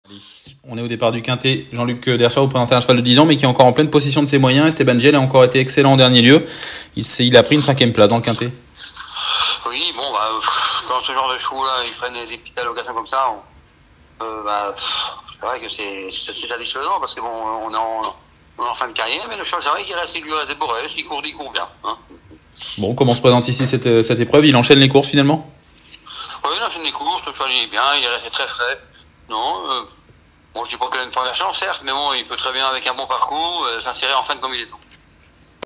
Jean Luc Dersoir, entraîneur de Esteban Jiel (mercredi 3 janvier à Paris Vincennes)
Jean-Luc Dersoir, entraîneur de Esteban Jiel (10) nous parle de ses chances dans le Quinté+, le Prix de Poitiers sur l’hippodrome de Paris-Vincennes le Mercredi 3 janvier 2024.